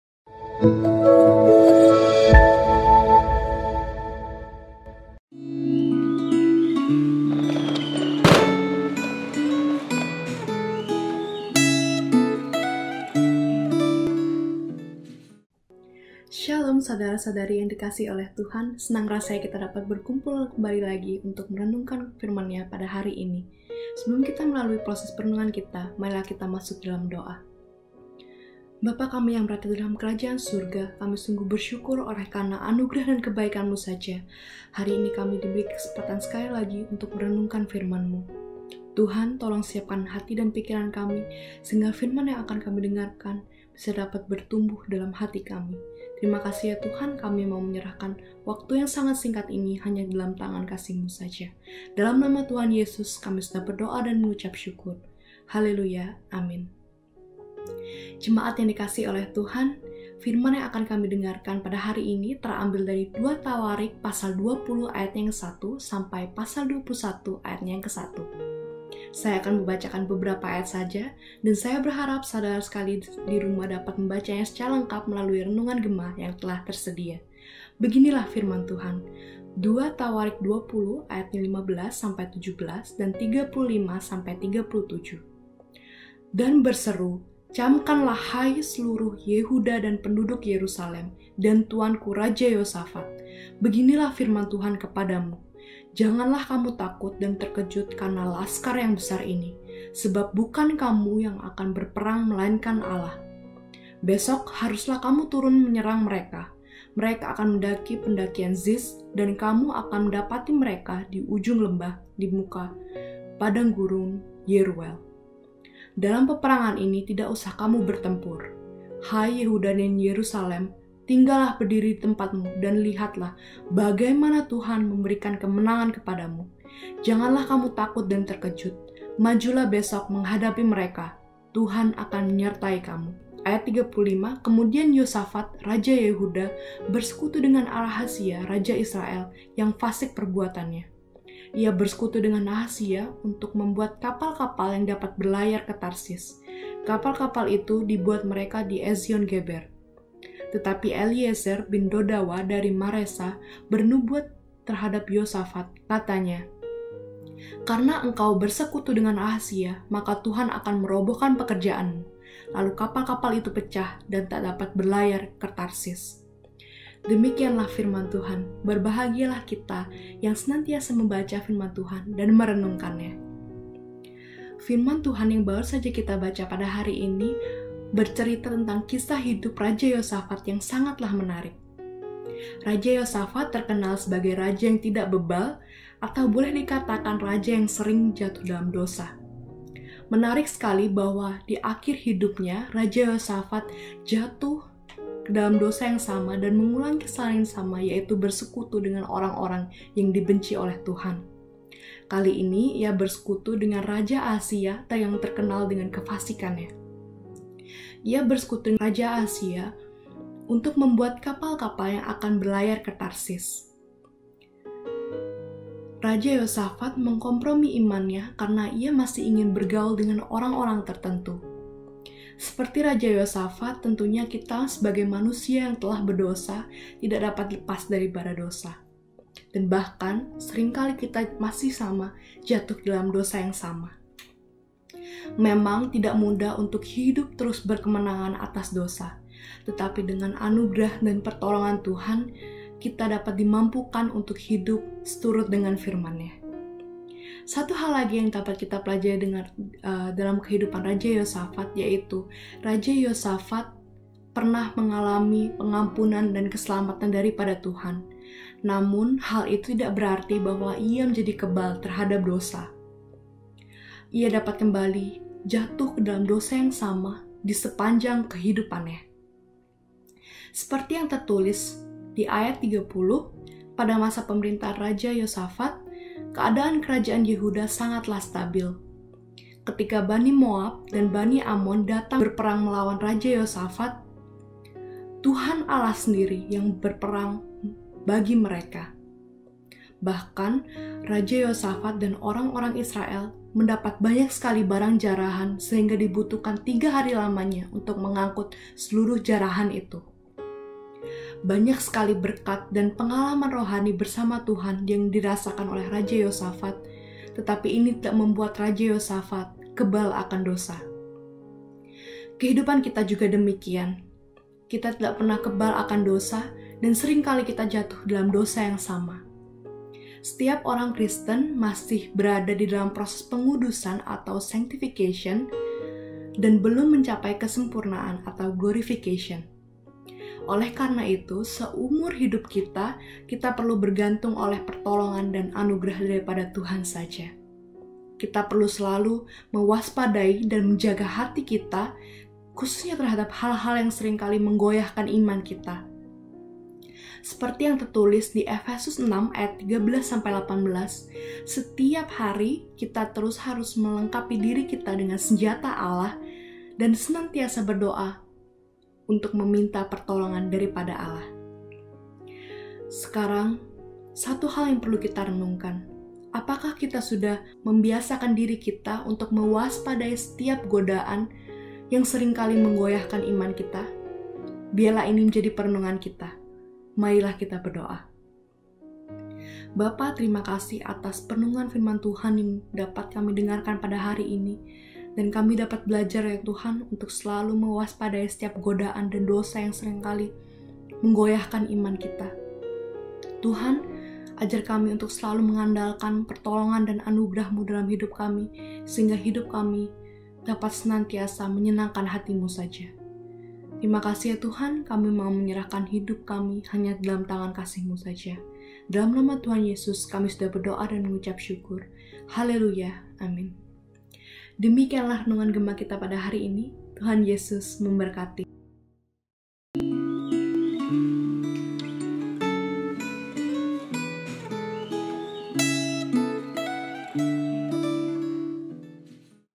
15 Februari 2022 - Tidak Kebal
Refleksi Gema 15 Februari 2022 - Tidak Kebal

Pembacaan: 2 Tawarikh 20:1–21:1
Pembawa Renungan: Rosalind (SYD)